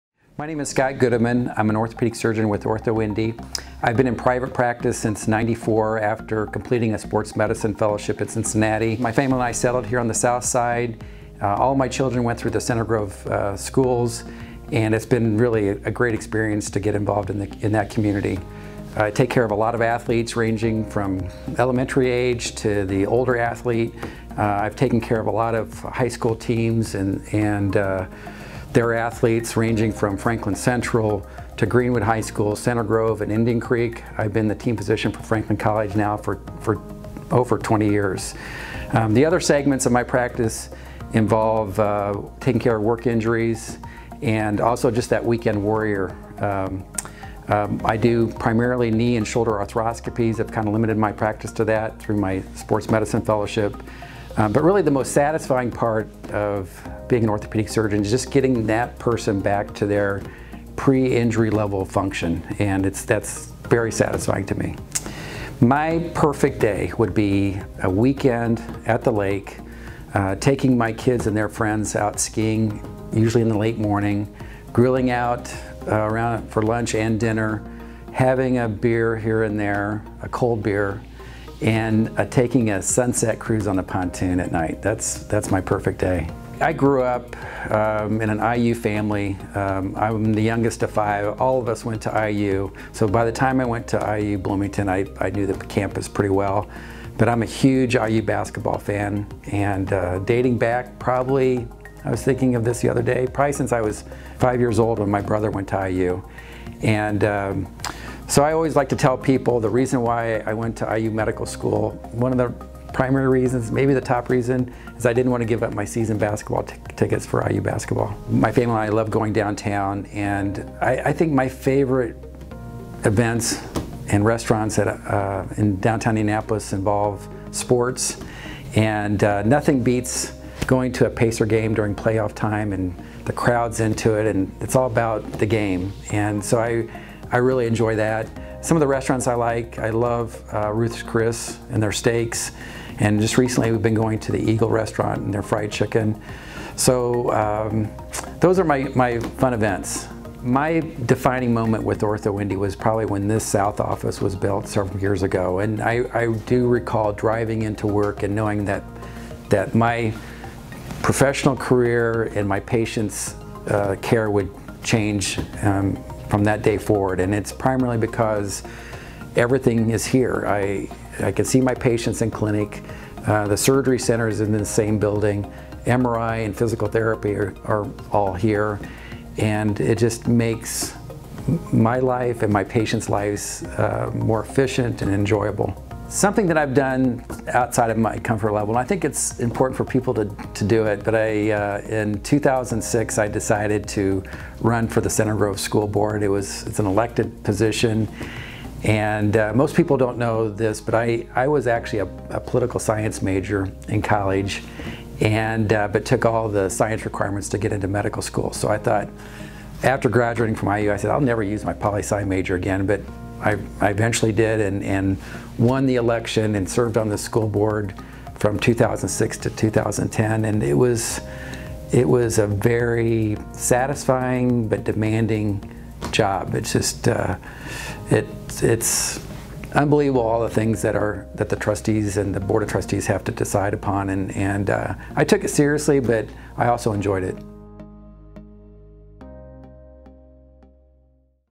OrthoIndy Spotlight | Dr. Scott Gudeman
Get to know Dr. Scott Gudeman as he discusses his career in sports medicine at OrthoIndy and what he enjoys doing outside of his professional career.